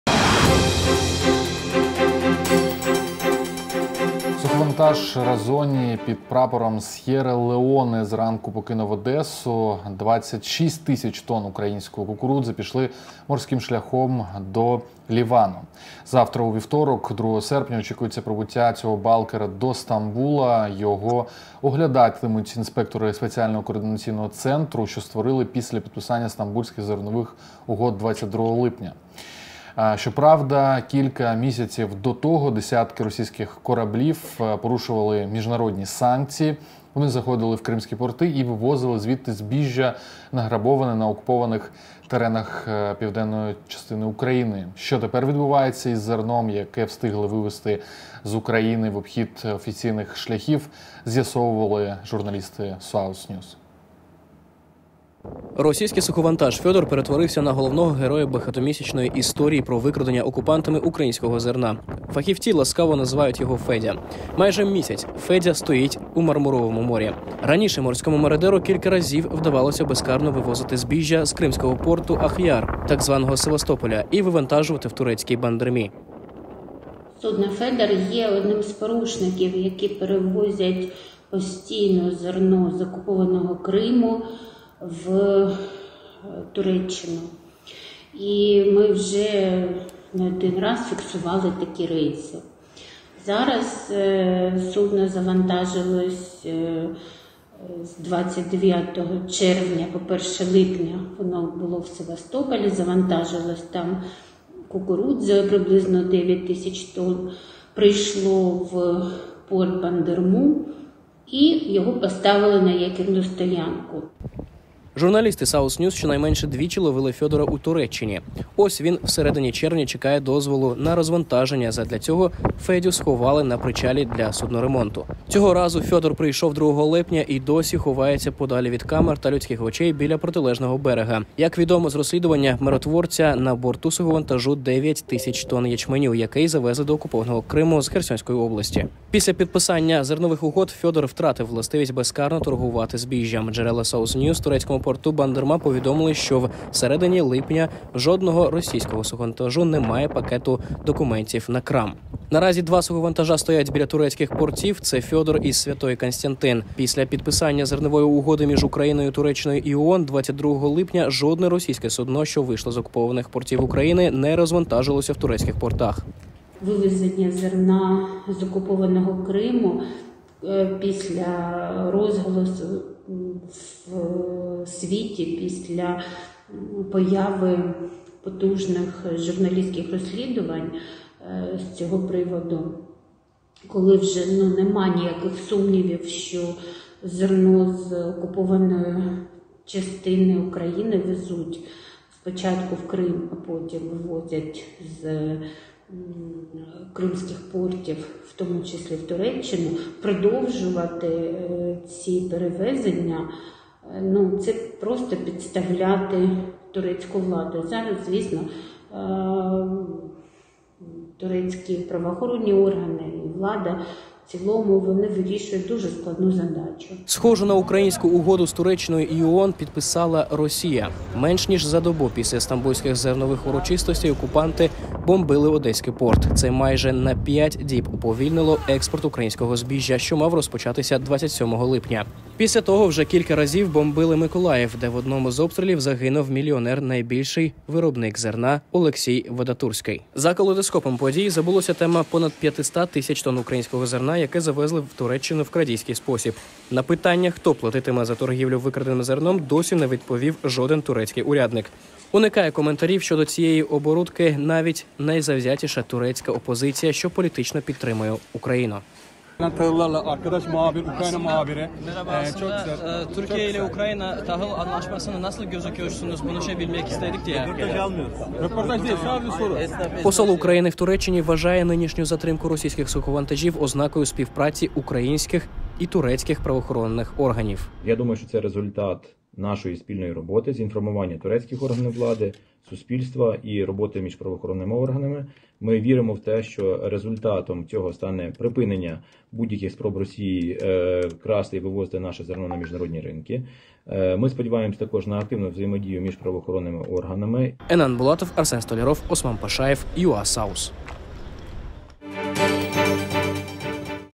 ЩО ВІДБУВАЄТЬСЯ З ЗЕРНОМ, ЯКЕ ВИВОЗЯТЬ З УКРАЇНИ
Україна почала офіційний експорт зерна згідно стамбульских угод. Про долю вкраденого Москвою зерна мовчать як в Києві так в Стамбулі.

Читайте наші відповіді на найважливіші для вас питання на нашому сайті